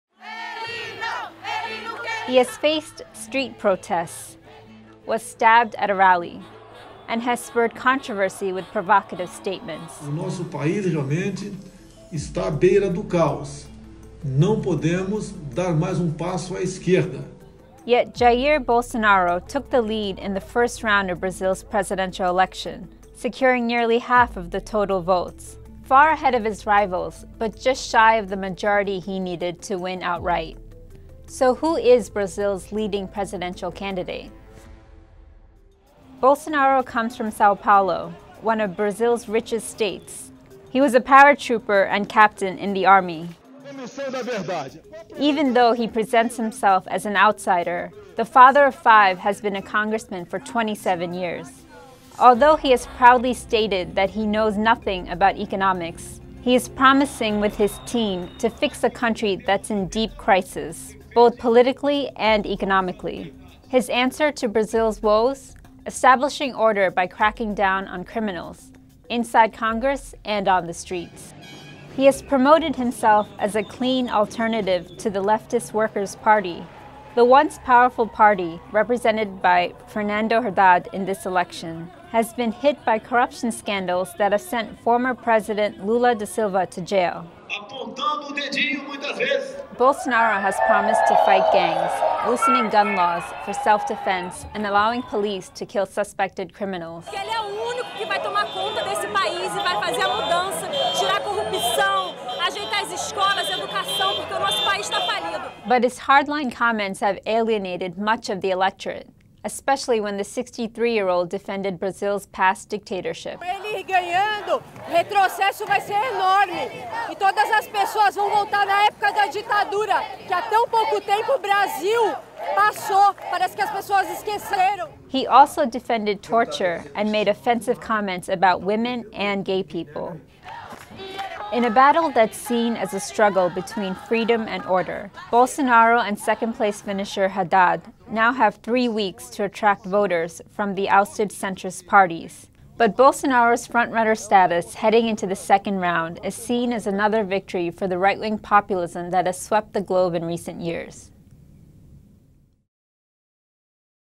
Brazil's Election: Who Is First-Round Winner Jair Bolsonaro?
Paratrooper-turned-politician Jair Bolsonaro won the first round of Brazil's election. His statements on dictatorship, torture, women and gay people make him a divisive candidate for the presidency. Photo: Getty